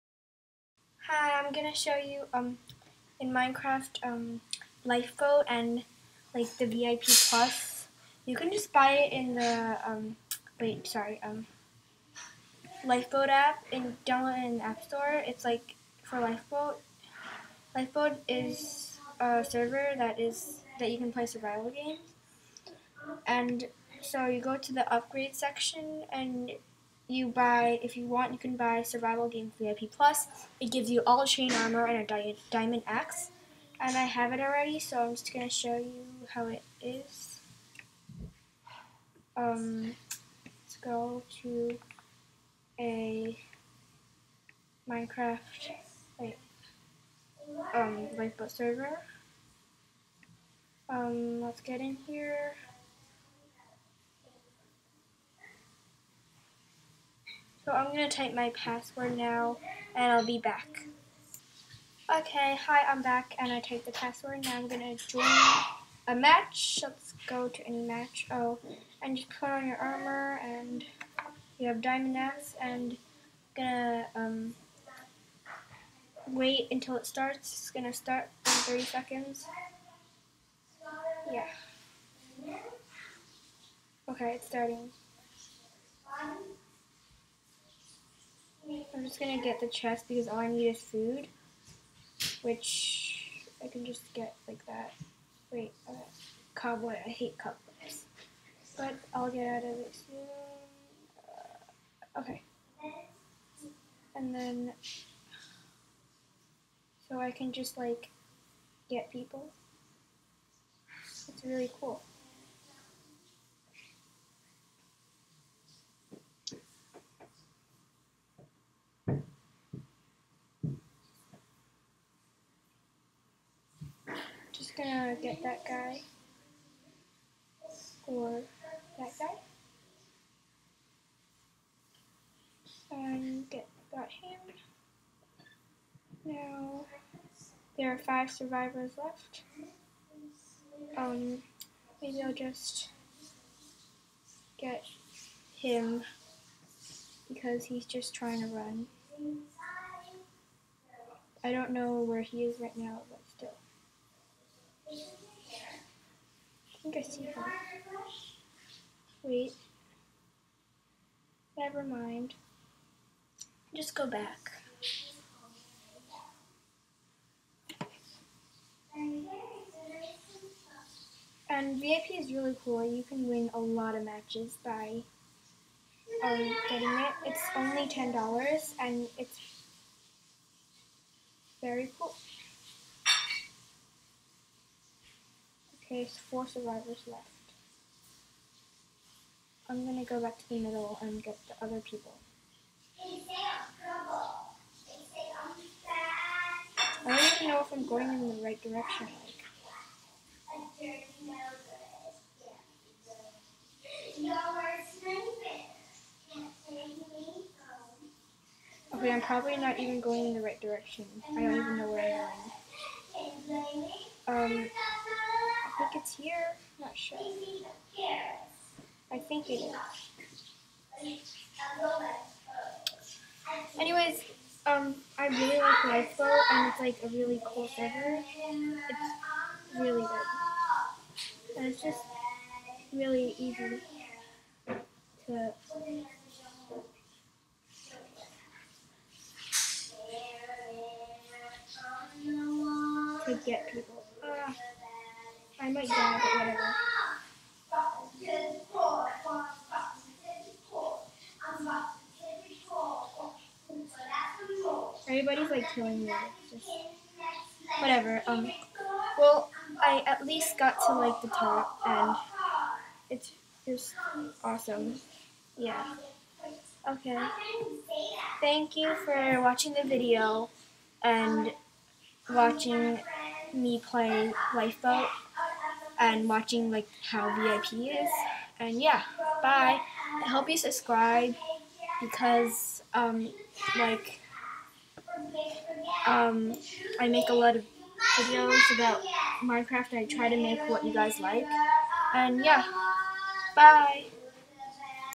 Lifeboat VIP+ is awesome!
Please watch te video to see how lifeboat VIP+ is. Here is the lifeboat survival games server IP: play.lbsg.net